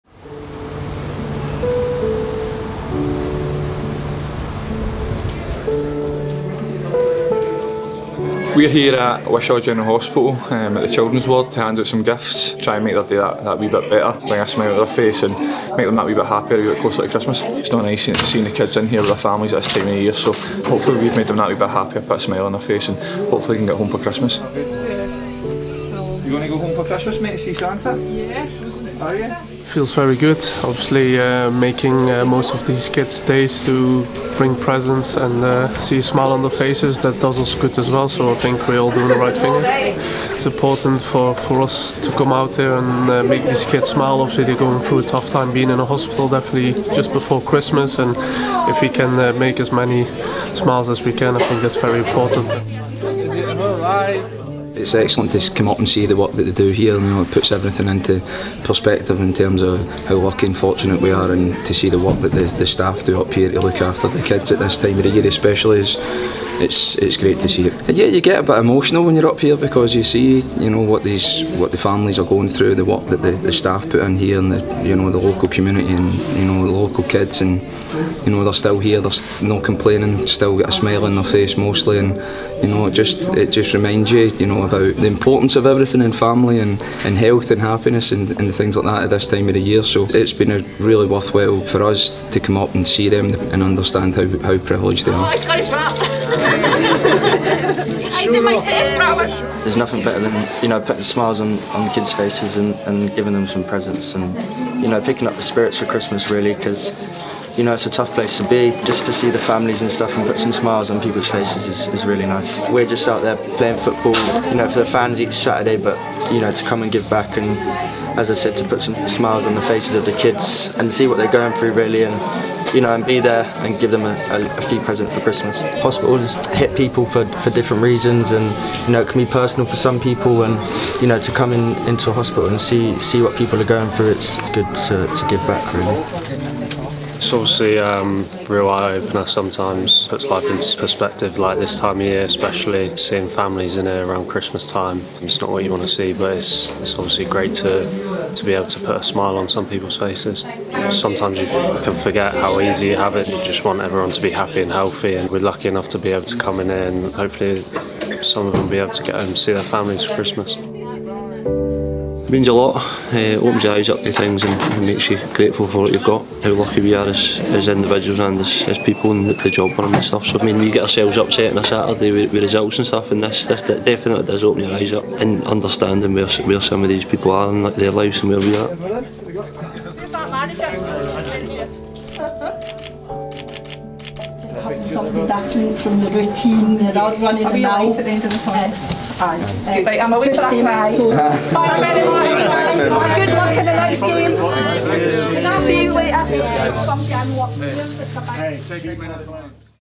Spreading the festive cheer // Motherwell players visit children's ward in Wishaw Hospital
At a time where you cherish family and friends, not everyone is looking forward to the festive period.

Motherwell players paid a visit to the children's ward at Wishaw General Hospital to say hello to the kids, hand out presents that were kindly donated by the club, Motherwell Football Club Community Trust and club partners.

Meeting patients and staff, all the players got a reminder of the privileged position that they are in and hoped they had given some happiness and festive cheer to the children who need it most.

Manager Steven Hammell and assistant manager Brian Kerr also popped along to see the excellent work the staff do at Wishaw General.